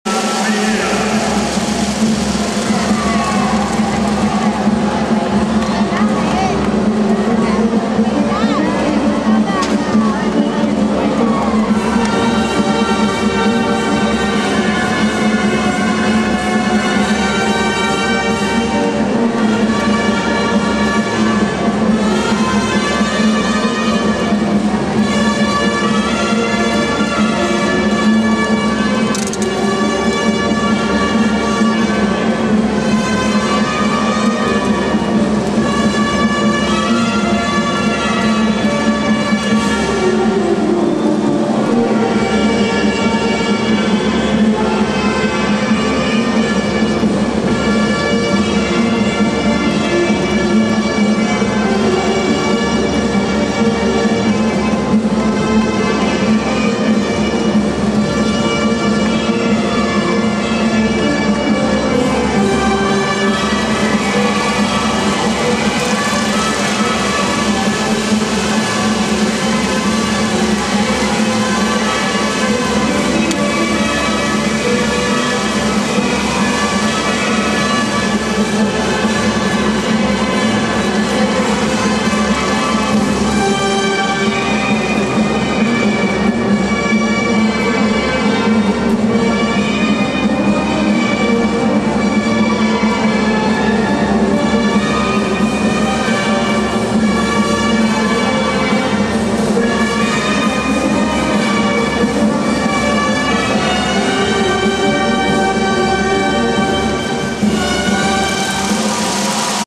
Segunda Final Categoria Adultos - Cinthya VArgas y Miguel Sandoval
L CONCURSO NACIONAL DE MARINERA
Organizado por el Club Libertad de Trujillo
BODAS DE ORO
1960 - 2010

Segunda Final
Categoria Adultos
Cinthya VArgas y Miguel Sandoval

Coliseo Gran Chimu
Trujillo - Perú